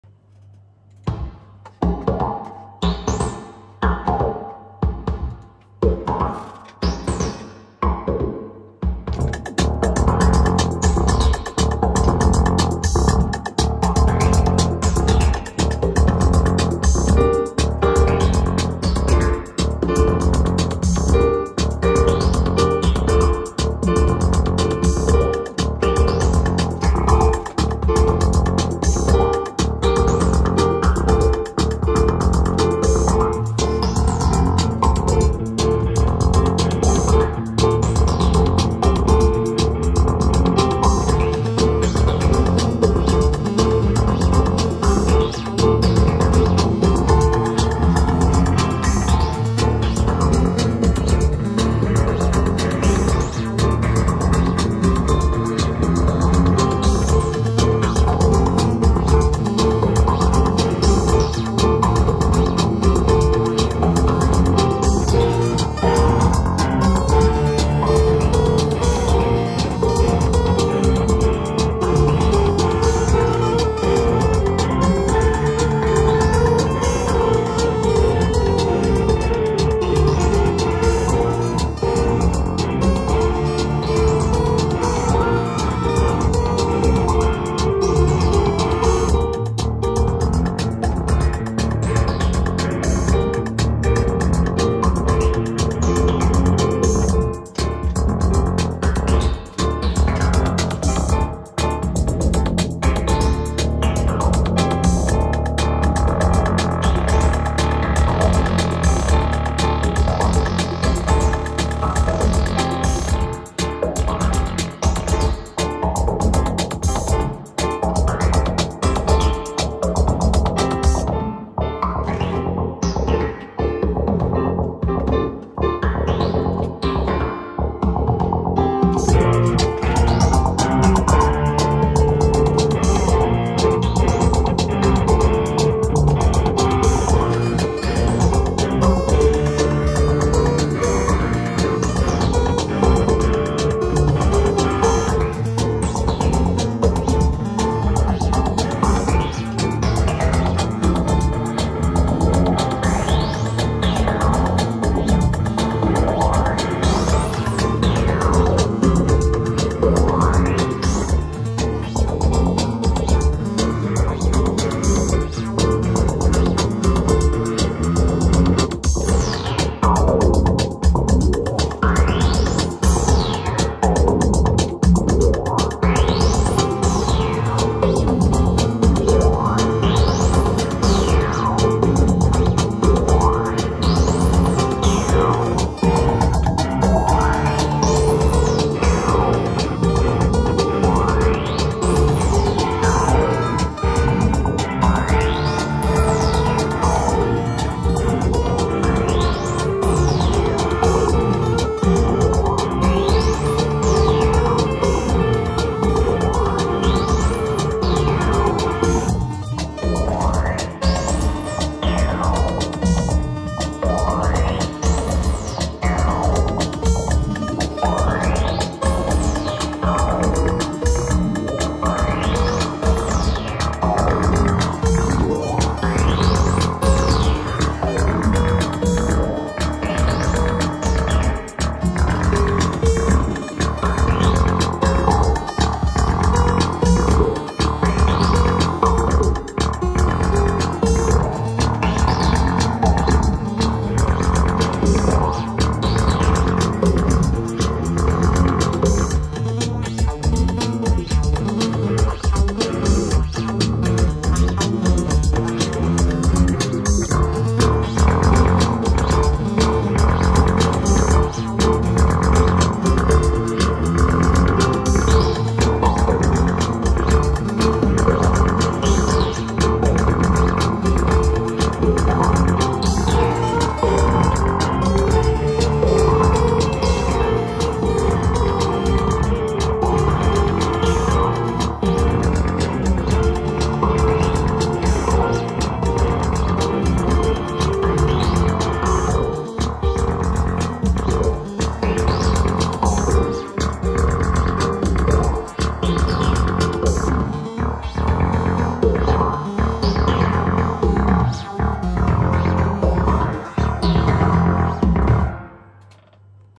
Eclipsing Myself
Just left the town for a long weekend, so no arturia akaimpclive2 or moog gear but still loopypro and also some piano, behringercrave and behringeredge as well as a modularsynth and a guitar.